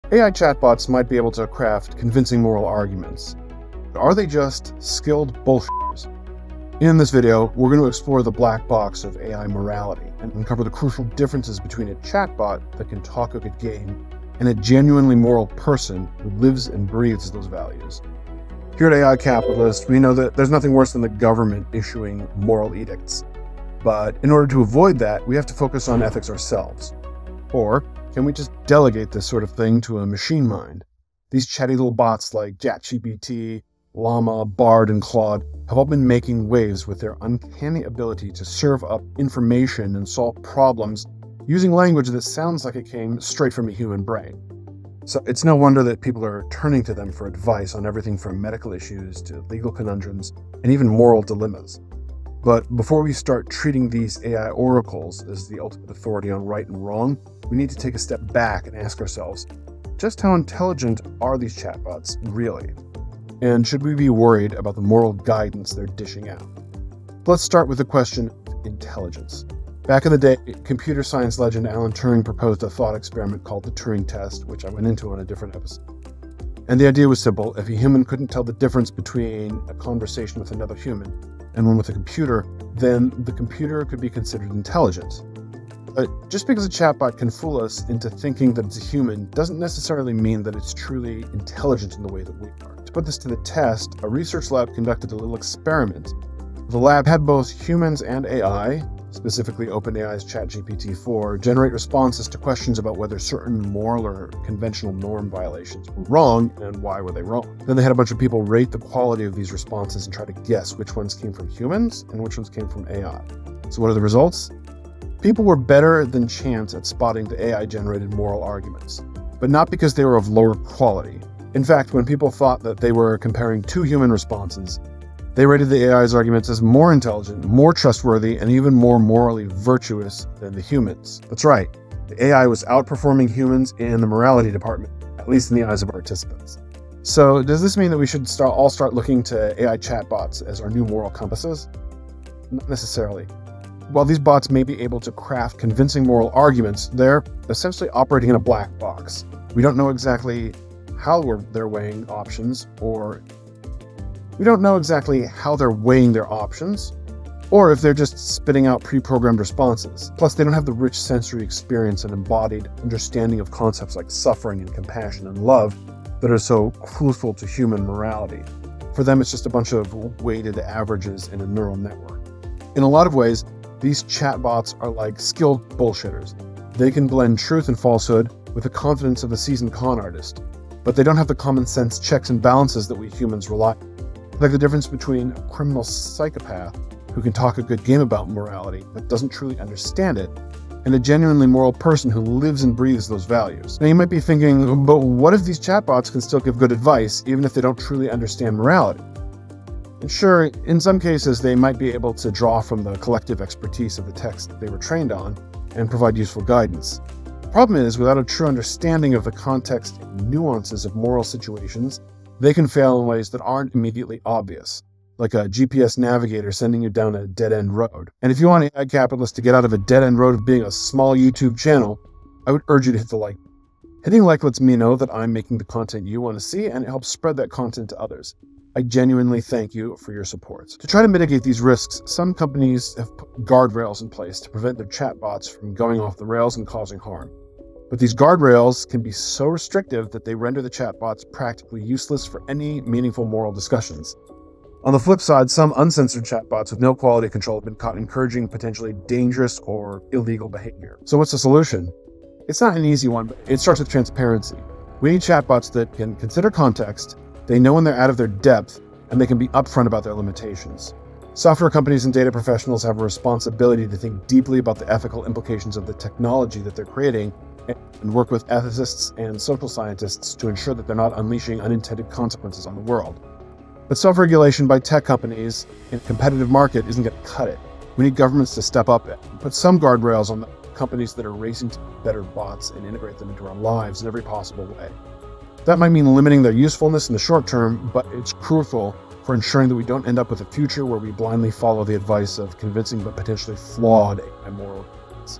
Why AI Chatbots Are the Ultimate Moral Manipulators
In this eye-opening episode, we dive deep into the murky waters of AI morality and expose the shocking truth that tech giants don't want you to know. Can we really trust chatbots like ChatGPT, LLaMA, Bard, and Claude to guide our moral compass, or are they just skilled charlatans with a knack for convincing rhetoric?
We put these AI oracles to the test in a groundbreaking experiment called the Moral Turing Test, pitting their responses against those of real humans. The results will leave you questioning everything you thought you knew about AI intelligence and its implications for our ethical decision-making.
But the rabbit hole goes deeper. We explore the black box of AI morality, revealing the hidden dangers and unintended consequences that arise when we blindly follow the advice of these convincing but potentially flawed machines. From uncensored chatbots encouraging dangerous behavior to the lack of transparency in AI development, we uncover the disturbing reality behind the illusion of AI ethics.
Are we on the brink of a terrifying future where AI replaces human moral judgment? Or can we harness the power of these technologies to enhance our ethical reasoning while mitigating the risks? We delve into the crucial role of governments, tech companies, and individuals like you in shaping the path forward.